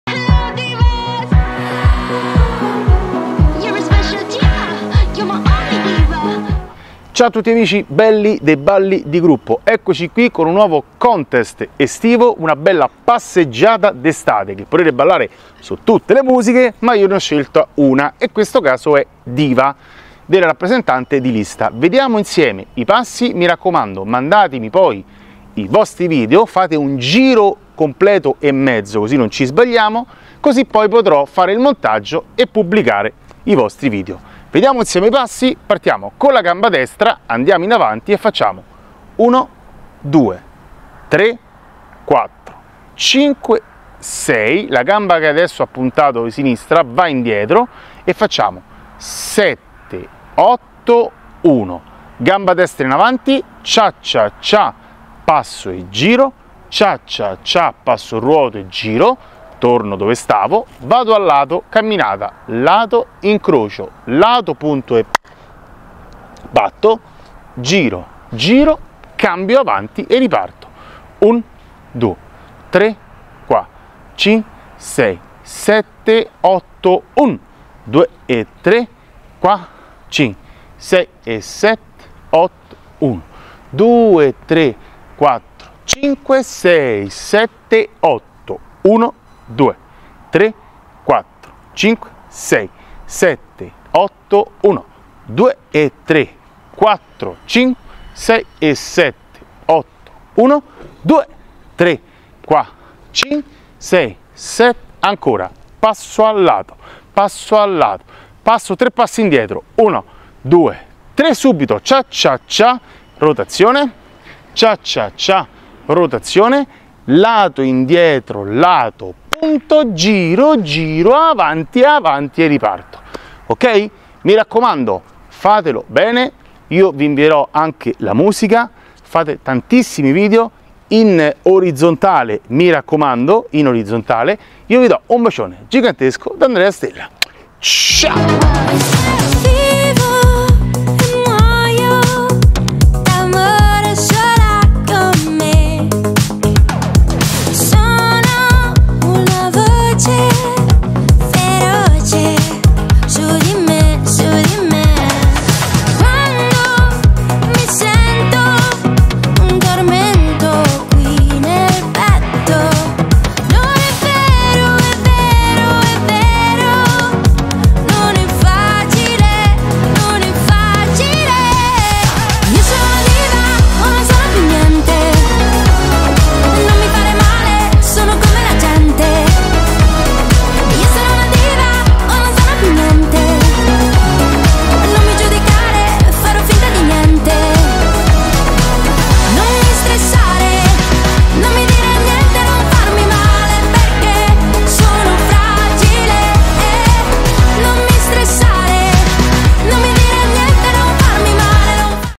DIVA || CONTEST || TUTORIAL E BALLO DI SPALLE || ANDREA STELLA || BALLI DI GRUPPO 2022 ||PASSEGGIATA
DIVA CONTEST - PASSEGGIATA D'ESTATE

Semplice mono-sequenza ballabile su tutte le musiche di Passeggiata.

TUTORIAL E BALLO DI SPALLE

Per partecipare al contest:

Registrate il vostro video, se siete ALMENO in DUE PERSONE (no video singoli).

Fate solo un giro completo e mezzo

Video in ORIZZONTALE

Inviami il video su TELEGRAM, entro giovedì 28 luglio

Per informazioni scrivimi al 328/6213033